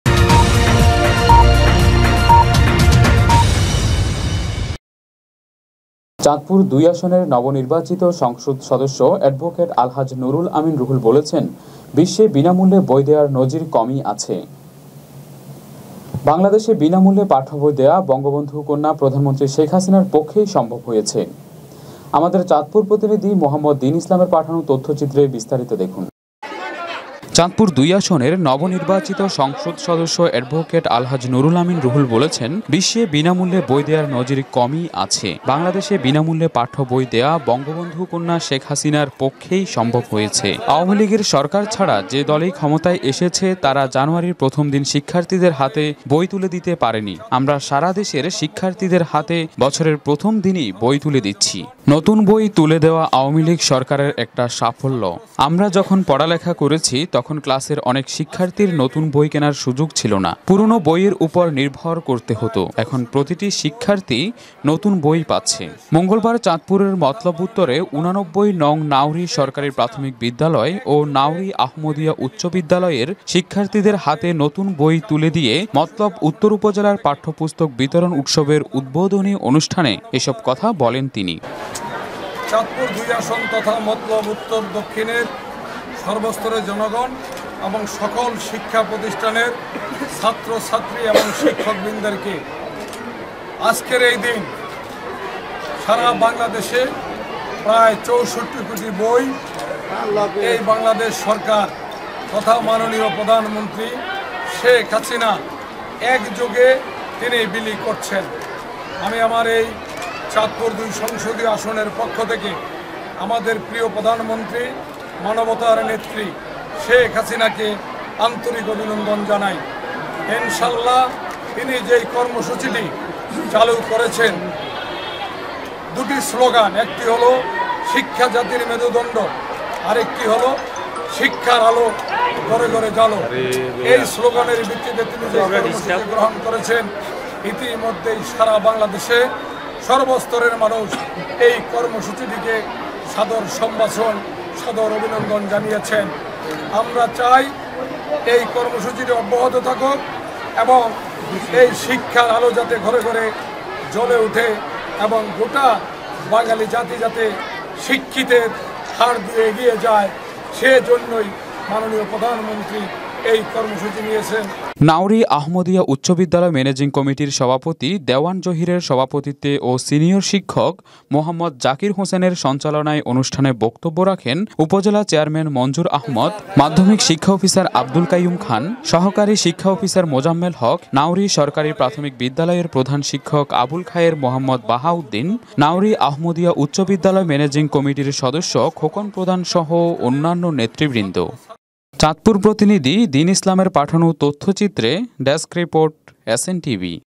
SNTV/ পাঠ্য বই উৎসবে নবনির্বাচিত সংসদ সদস্য নুরুল আমিন রুহুল/MOTLOB UTTAR CHANDPUR/BANGLA NEWS/2019
মতলব উত্তরে পাঠ্য বই উৎসবে নবনির্বাচিত সংসদ সদস্য নুরুল আমিন রুহুল বিনামূল্যে পাঠ্যবই দেয়া শেখ হাসিনার পক্ষেই সম্ভব